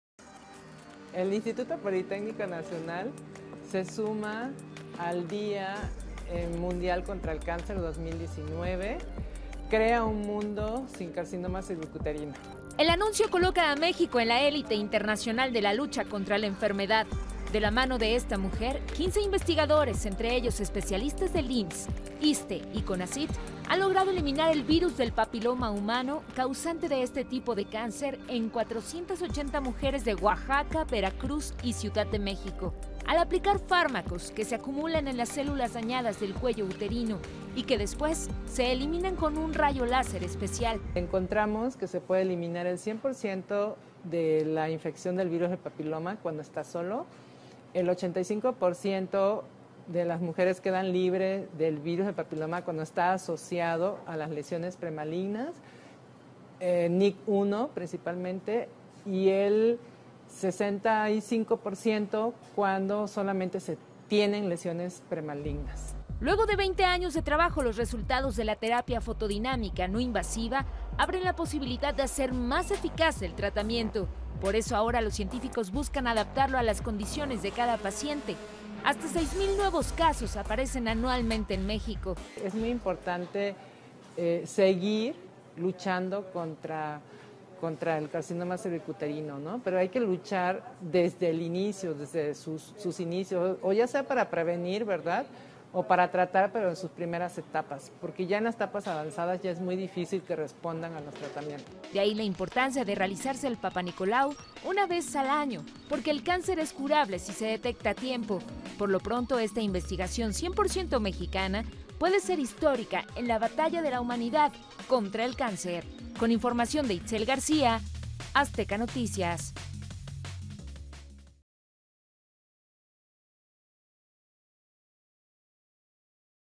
En México eliminan el virus del papiloma humano en 480 mujeres - Noticias SIN
MÉXICO.- Especialistas mexicano han logrado eliminar el virus del papiloma humano, causante de cáncer, en 480 mujeres de Oaxaca, Veracruz y ciudad de México.